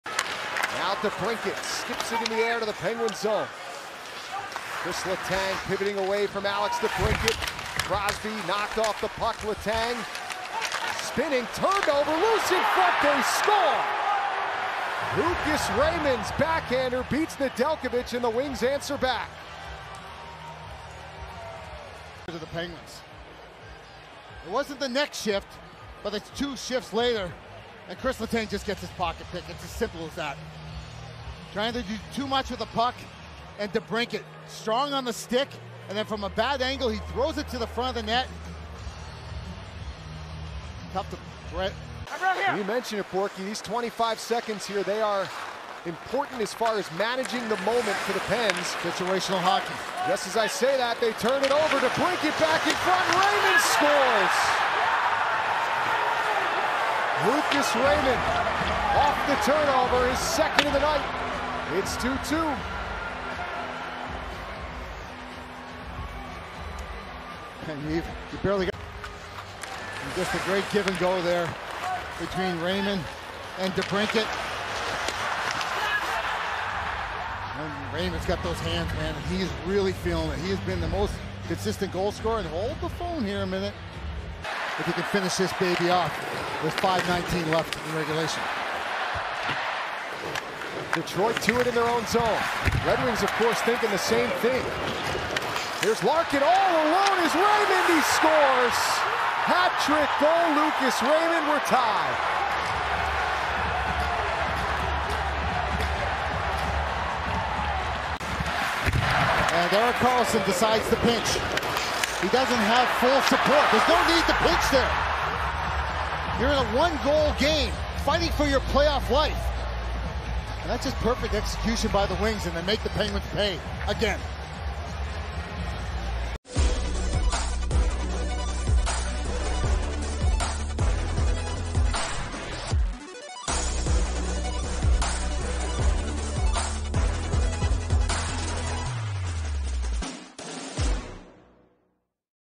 Lucas Raymond Scores Clutch Hat Trick vs. Penguins
Watch as Detroit Red Wings forward Lucas Raymond scores his second career hat trick to help even the score against the Pittsburgh Penguins in one of the most consequential games of the season.